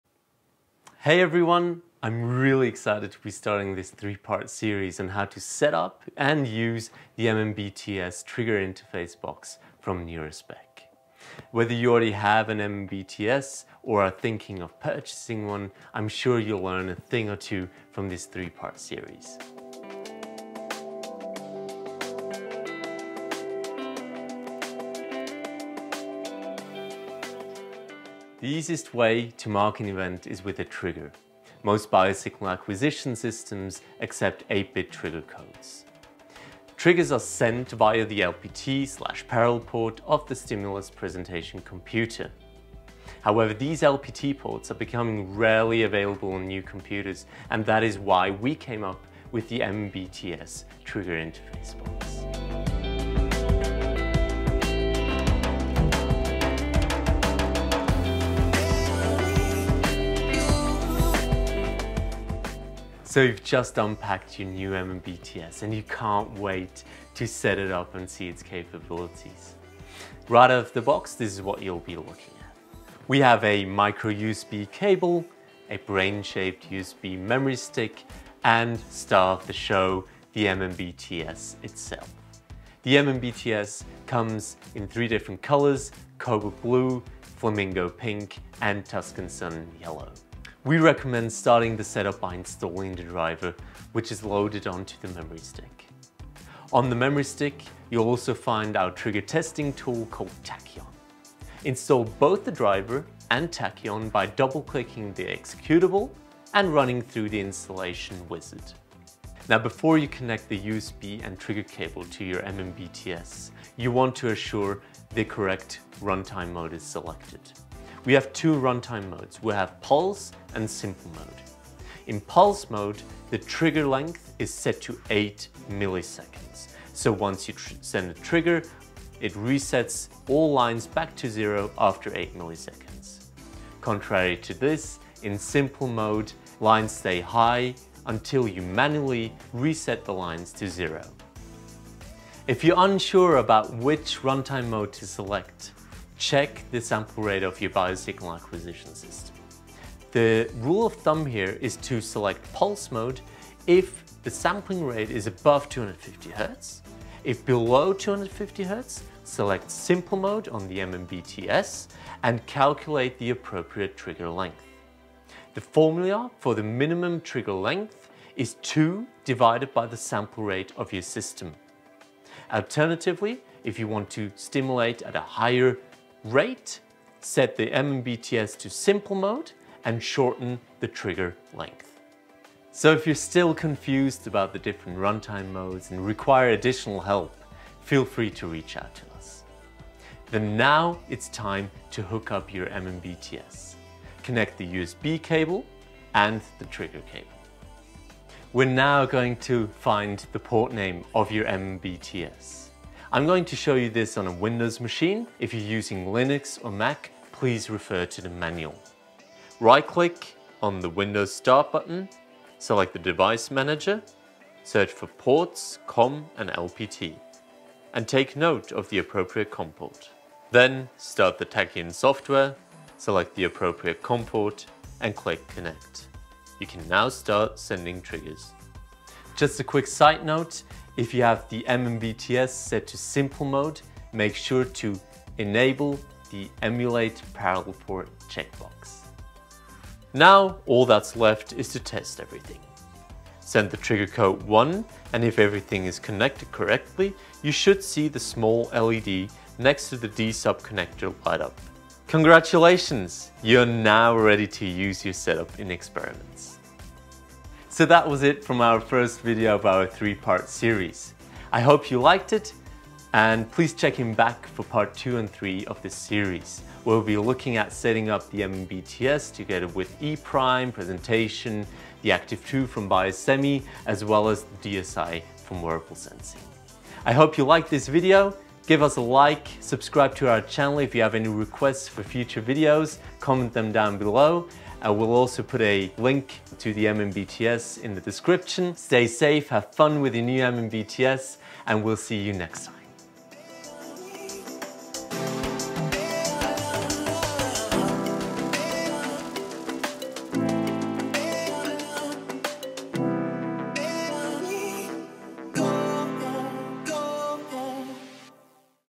MMBT-S (Part 1 of 3): Installation and Setup | NEUROSPEC AG
This is part 1 of our 3 series on how to install, set up, and use our MMBT-S trigger interface box.

In EEG experiments, timing is everything. As a researcher, you need to know the precise moment an event happens. This is why we here at NEUROSPEC came up with the MMBT-S trigger interface box. The MMBT-S allows for sending 8-bit trigger markers via the USB socket on your computer with sub-millisecond precision.

In this video you will learn:

What comes with your new MMBT-S
How to set it up and connect everything
How to send your first triggers
What would you like to see us do next?

NEUROSPEC AG
Stansstaderstrasse 10
CH - 6370 Stans - NW
SWITZERLAND